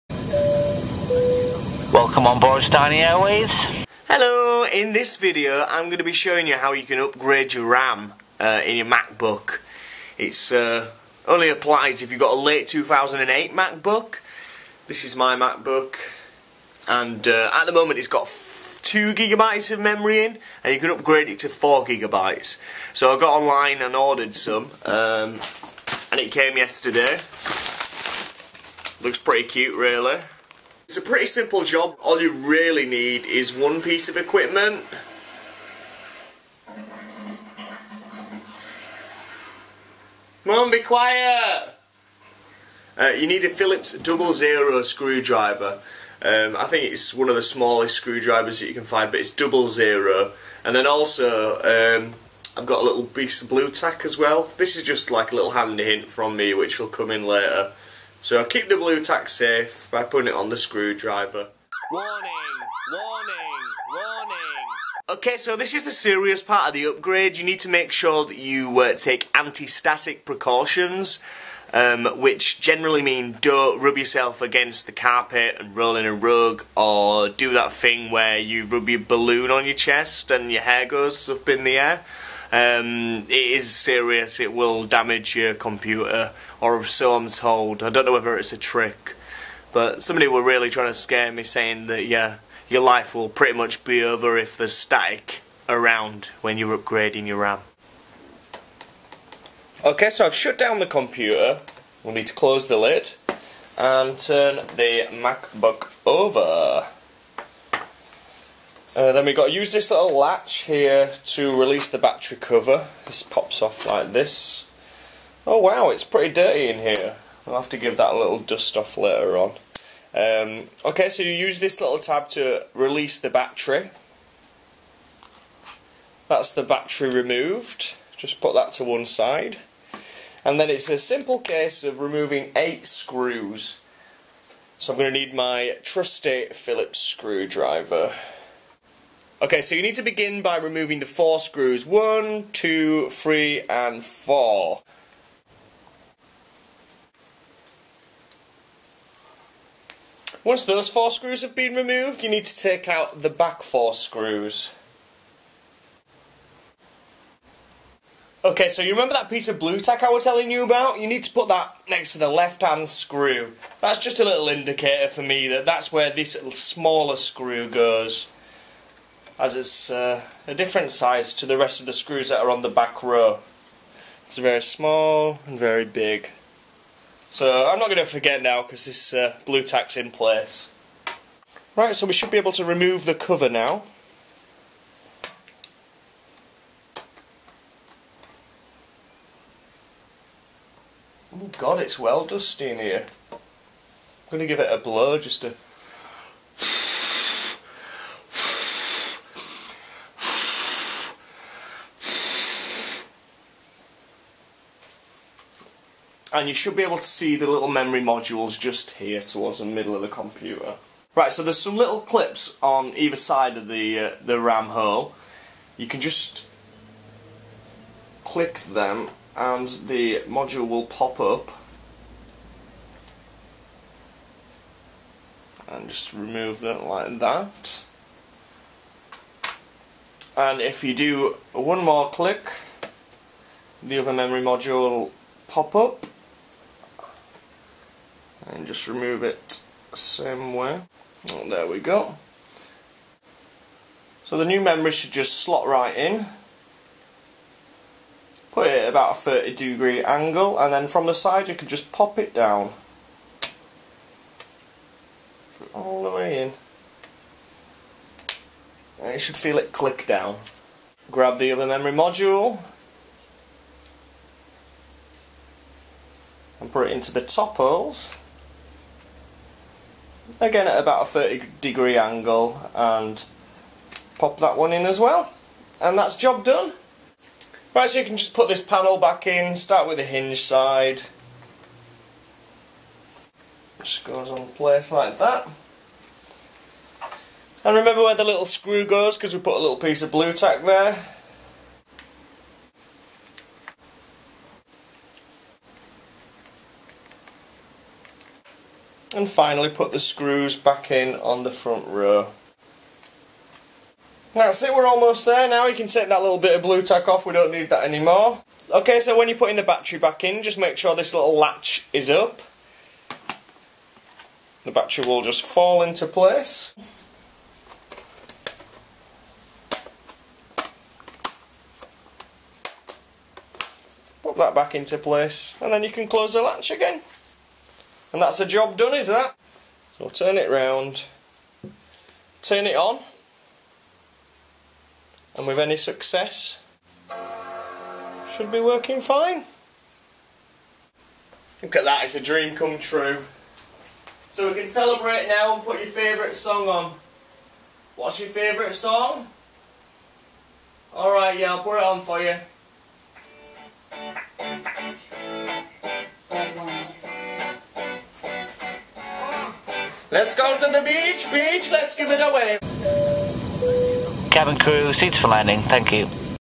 How to: Upgrade RAM in a Macbook (Late 2008)
In this video I will show you how you can install R.A.M in a late 2008 aluminum Mac Book. The Mac's RAM can be doubled from 2GB (Factory standard) to 4GB, this will give you potential to run more programs at once and also increase processing speed. I mainly use my Macbook for video editing and designing using adobe illustrator and I have found it speeds things up rather nicely.